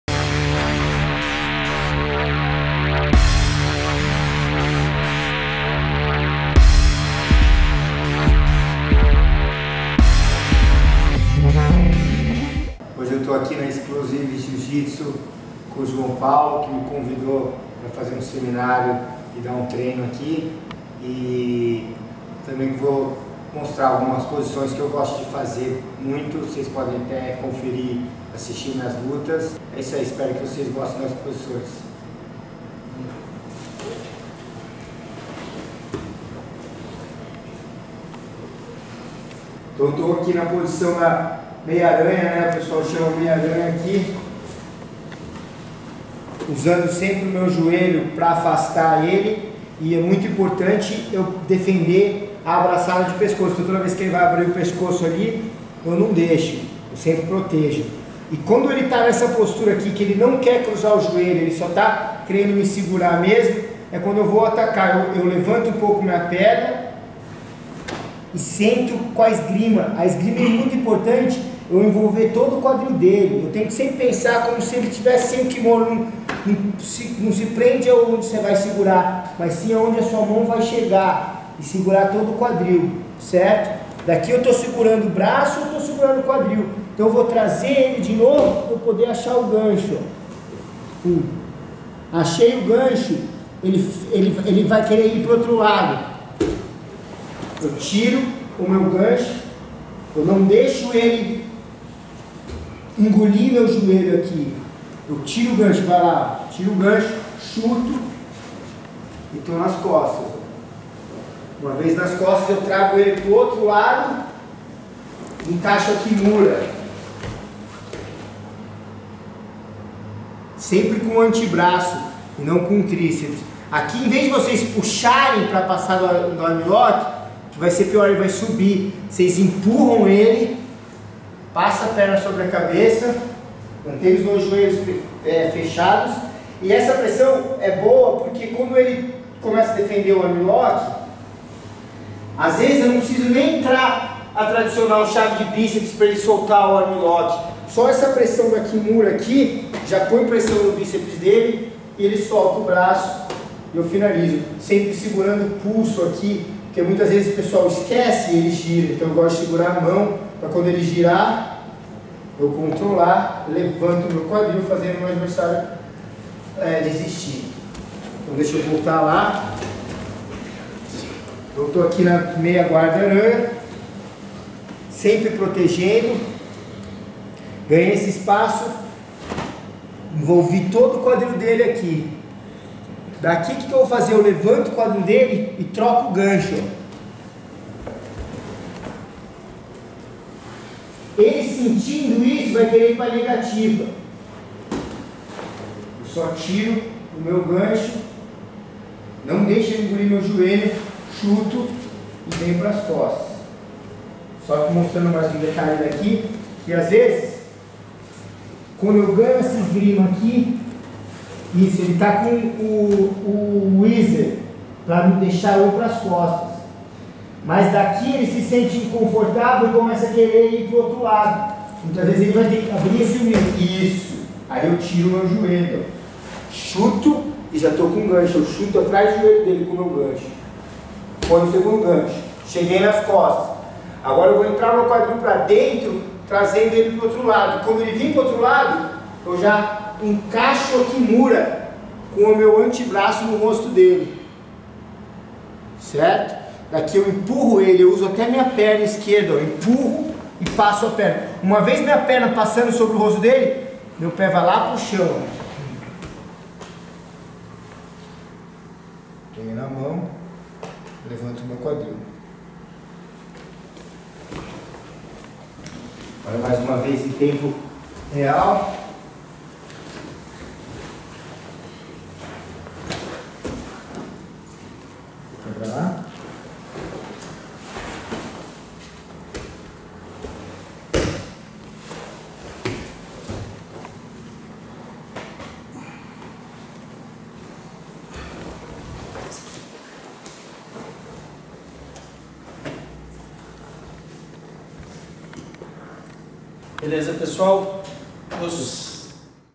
Lucas Leite - Taking the Back from Half Guard to Armlock - Essence Of Jiu-Jitsu
Lucas Leite and João Paulo Bertuccelli.

• Posição :

- Pegada de costas partindo da meia guarda por baixo, finalizando no armlock.

• Position :

- Taking the back from Half Guard, and then attacking with a armbar.